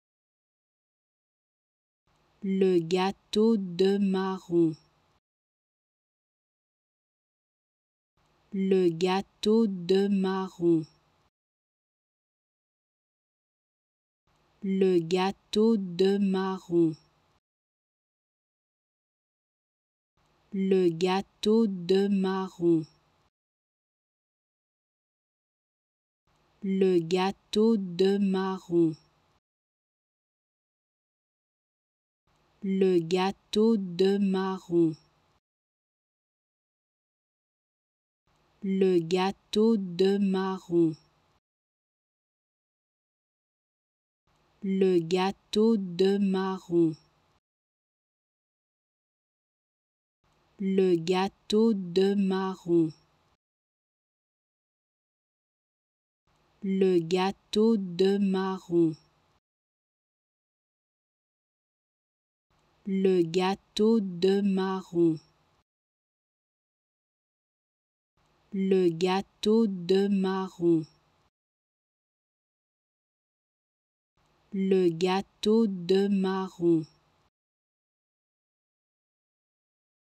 How to Pronounce ''Le gâteau de marron'' (The chestnut cake) Correctly in French
Learn how to say and properly pronounce ''Le gâteau de marron'' in French with this free pronunciation tutorial. Apprenez à prononcer des mots en français grâce à nos tutoriels.